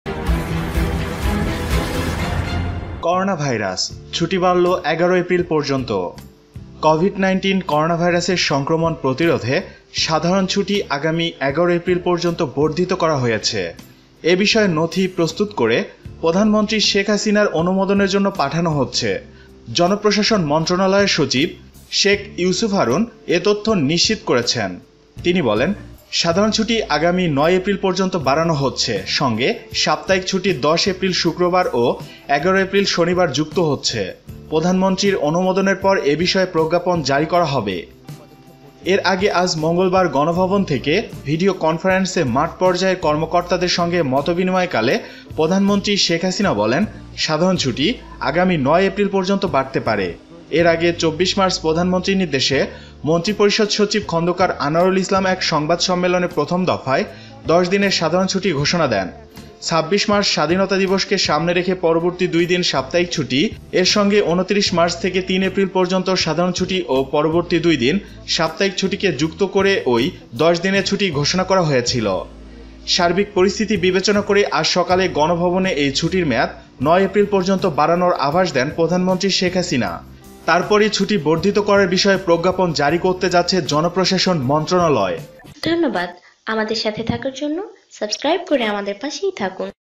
এইমাত্র পাওয়া-ছুটি বাড়ল ১১ এপ্রিল পর্যন্ত ।। Sheikh Hasina ।। Bangladesh ।। Bangla News !
@MH !! এইমাত্র পাওয়া ছুটি বাড়ল ১১ এপ্রিল পর্যন্ত ।। Sheikh Hasina ।। Bangladesh ।। Bangla News !

সাধারণ ছুটি আগামী ১১ এপ্রিল পর্যন্ত বর্ধিত করা হয়েছে। এ বিষয়ে নথি প্রস্তুত করে প্রধানমন্ত্রী শেখ হাসিনার অনুমোদনের জন্য পাঠানো হচ্ছে। জনপ্রশাসন মন্ত্রণালয়ের সচিব শেখ ইউসুফ হারুন বাংলাদেশ প্রতিদিনকে এ তথ্য নিশ্চিত করেছেন।

তিনি বলেন, সাধারণ ছুটি আগামী ৯ এপ্রিল পর্যন্ত বাড়ানো হচ্ছে। সঙ্গে সাপ্তাহিক ছুটি ১০ এপ্রিল (শুক্রবার) ও ১১ এপ্রিল (শনিবার) যুক্ত হচ্ছে। প্রধানমন্ত্রীর অনুমোদনের পর এ বিষয়ে প্রজ্ঞাপন জারি করা হবে।

এর আগে আজ মঙ্গলবার গণভবন থেকে ভিডিও কনফারেন্সে মাঠ পর্যায়ের কর্মকর্তাদের সঙ্গে মতবিনিময়কালে প্রধানমন্ত্রী শেখ হাসিনা বলেন, সাধারণ ছুটি আগামী ৯ এপ্রিল পর্যন্ত বাড়তে পারে।

এর আগে ২৪ মার্চ প্রধানমন্ত্রীর নির্দেশে  মন্ত্রিপরিষদ সচিব খন্দকার আনোয়ারুল ইসলাম এক সংবাদ সম্মেলনে প্রথম দফায় ১০ দিনের সাধারণ ছুটির ঘোষণা দেন। ২৬ মার্চ স্বাধীনতা দিবসকে সামনে রেখে পরবর্তী দুই দিন সাপ্তাহিক ছুটি, এর সঙ্গে ২৯ মার্চ থেকে ৩ এপ্রিল পর্যন্ত সাধারণ ছুটি ও পরবর্তী দুই দিন সাপ্তাহিক ছুটিকে যুক্ত করে ওই ১০ দিনের ছুটি ঘোষণা করা হয়েছিল।

সার্বিক পরিস্থিতি বিবেচনা করে আজ সকালে গণভবনে এই ছুটির মেয়াদ ৯ এপ্রিল পর্যন্ত বাড়ানোর আভাস দেন প্রধানমন্ত্রী শেখ হাসিনা।  তারপরই ছুটি বর্ধিত করার বিষয়ে প্রজ্ঞাপন জারি করতে যাচ্ছে জনপ্রশাসন মন্ত্রণালয়।